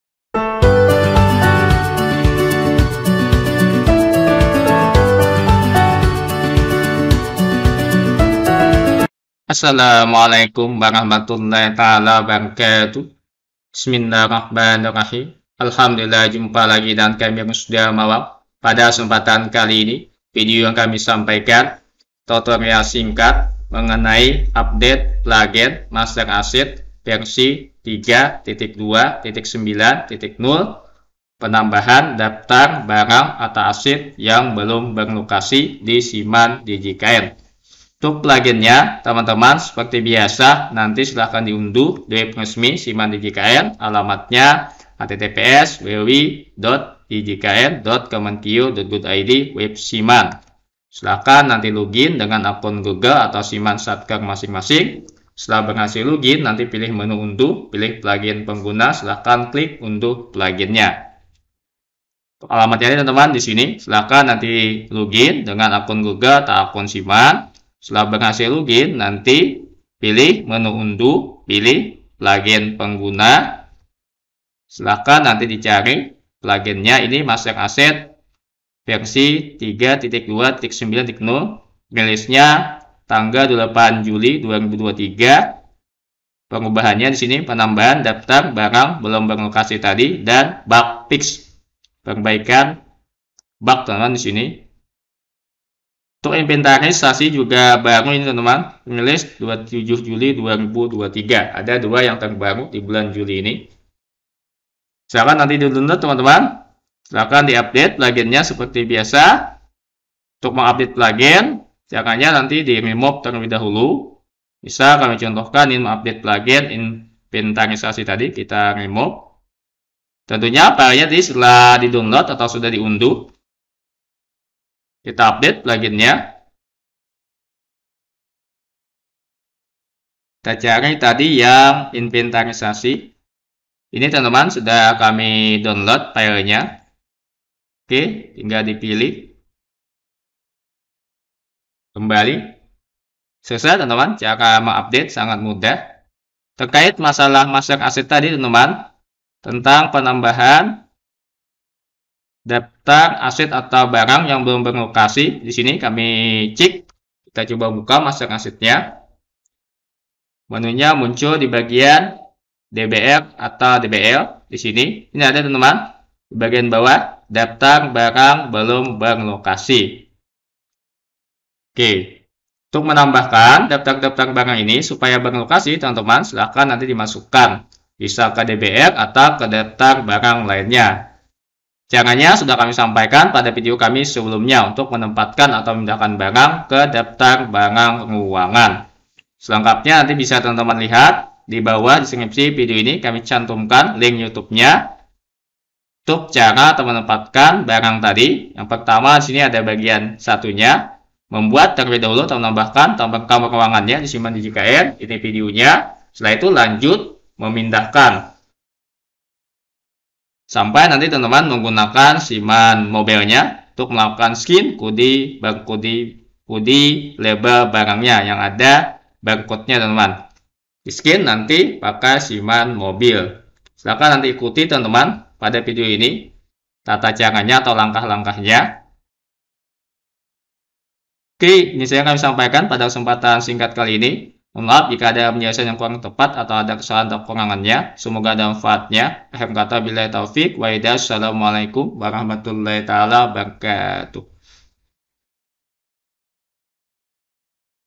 Penambahan daftar barang/aset belum berlokasi - Update Plugin Master Aset 3.2.9.0 di Siman DJKN
Update Plugin Master Aset  3.2.9.0  Penambahan daftar barang/aset belum berlokasi di Siman DJKN
1. Untuk Plugin silakan unduh di web resmi siman djkn di sini
2. Silahkan login dengan akun google atau siman satker
3. Setelah berhasil login pilih menu unduh
4. Pilih plugin pengguna
5. Silahkan klik unduh pluginnya.